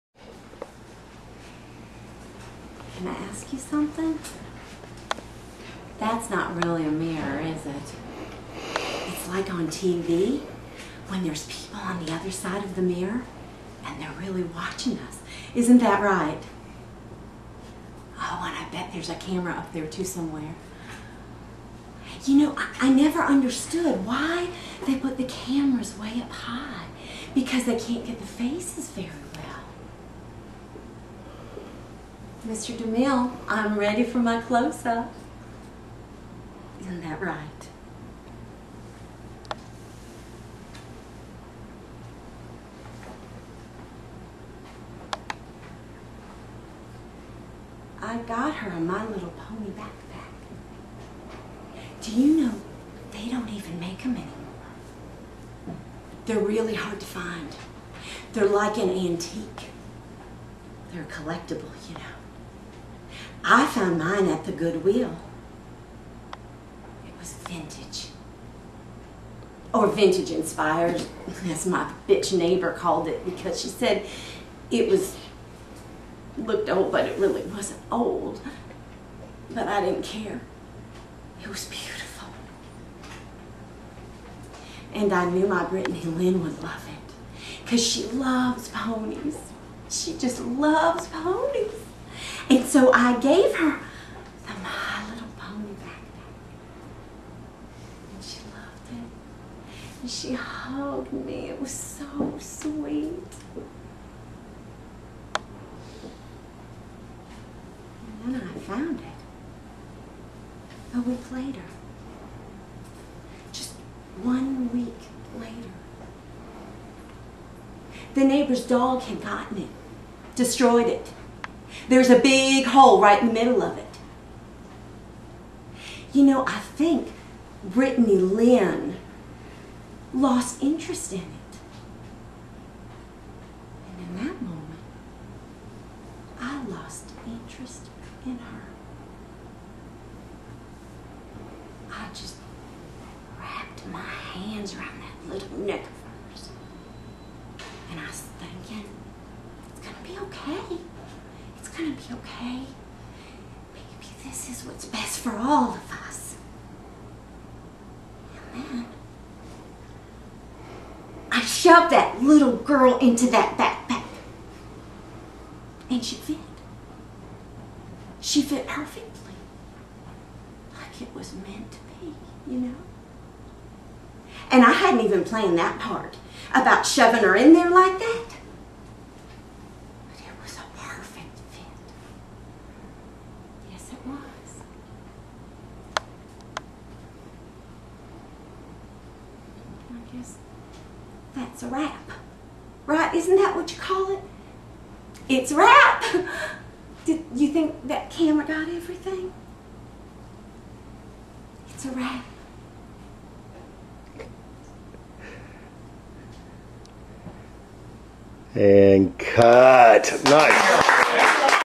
HEATHER P in "My Little Pony" - a Film Monologue written by Ben Taylor
THE BEN TAYLOR WORKSHOP, SUMMER 2013 - THE STUDIO KITCHEN SESSIONS, ATLANTA GA